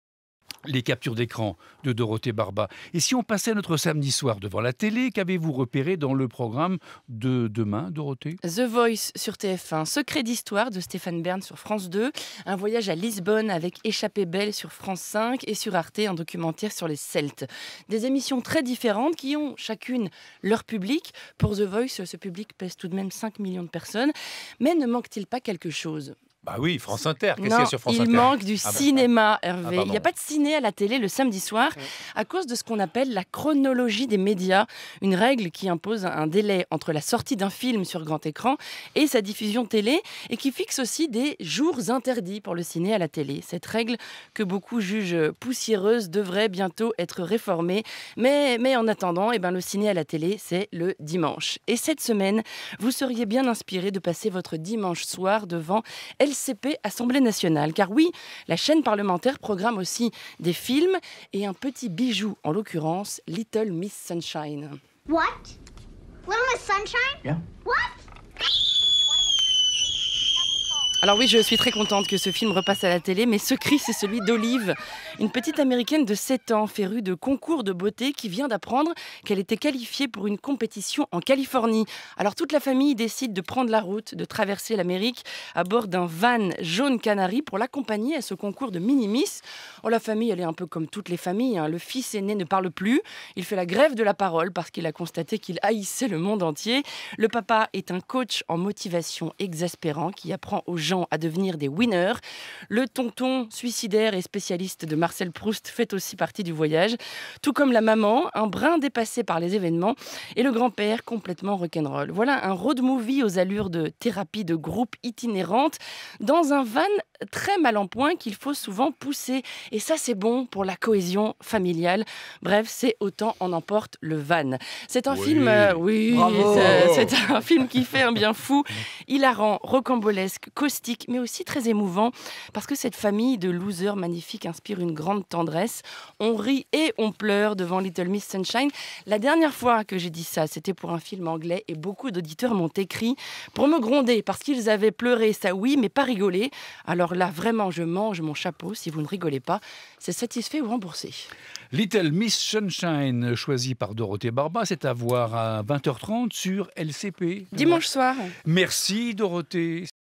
Little Miss Sunshine : autant en emporte le van - Capture d'écrans
A bord d'un combi mal en point, la famille Hoover traverse l'Amérique pour accompagner la petite dernière à un concours de beauté. Little Miss Sunshine, à voir dimanche soir sur LCP, est un road movie aux allures de thérapie de groupe itinérante. Hilarant et bourré de tendresse.

Captures d’écran de Dorothée Barba à 6h53 ( Capture d’écrans – 4 mai 2018)